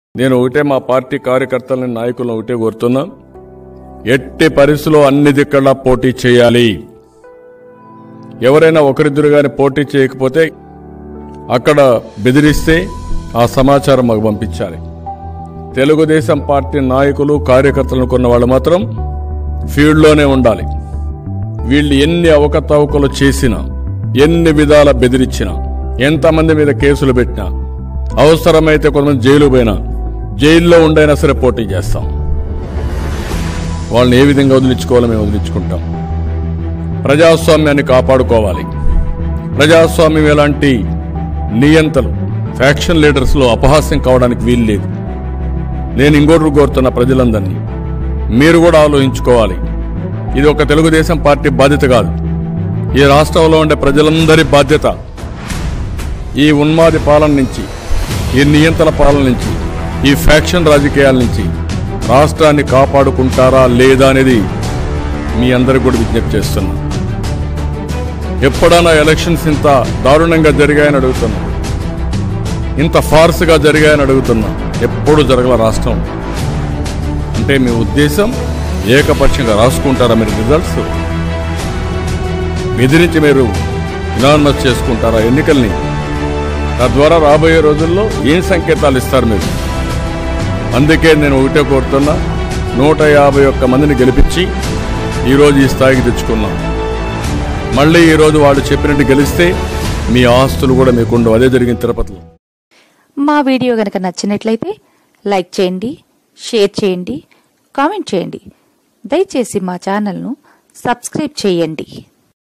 తెలుగుదేశం అన్ని స్థానాలలో పోటీ చేస్తుంది.
చంద్రబాబు సందేశం, తెలుగుదేశం అన్ని స్థానాలలో పోటీ చేస్తుంది.